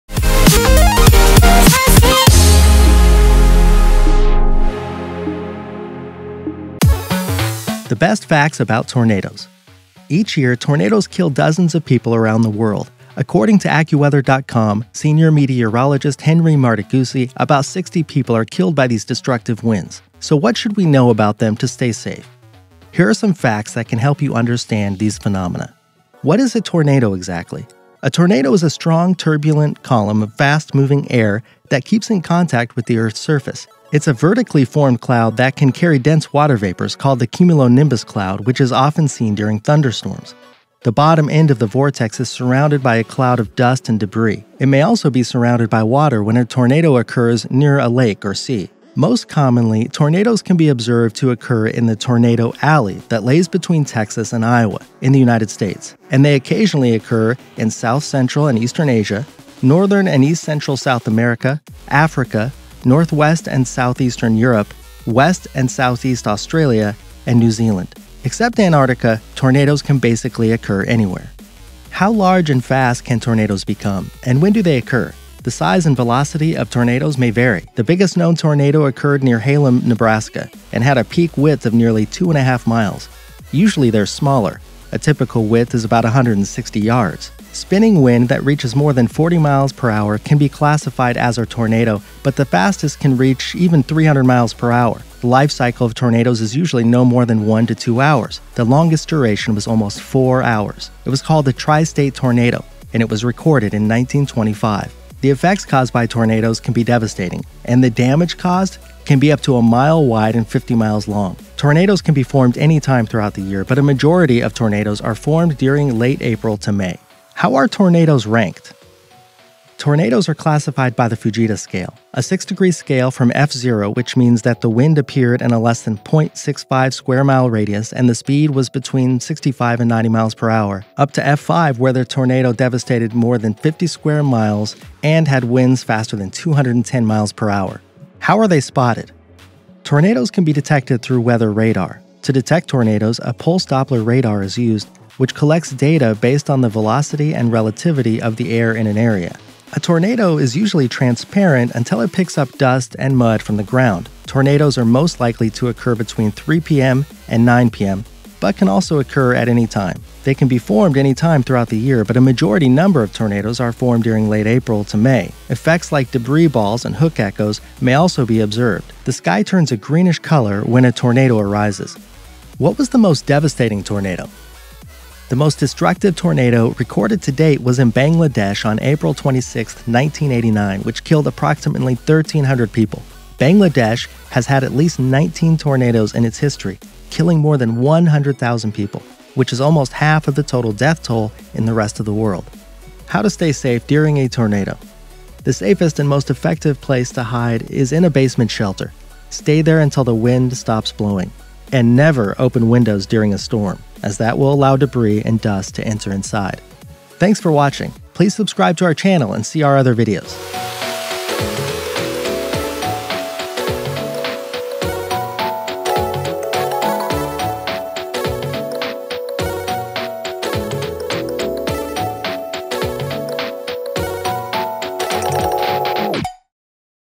The Best Facts About Tornadoes ★ Top Lists ★ FactsARMY ★★★
Top amazing facts about tornadoes (video).Tornado definition. What is a tornado exactly? What causes a tornado? Where is Tornado Alley located (pictures)?
How large, and fast, can tornadoes (storm,cyclone) become, and when do they occur? How are tornados ranked?How are they spotted?What was the most devastating tornado? How to stay safe during tornado?
All answers in this video.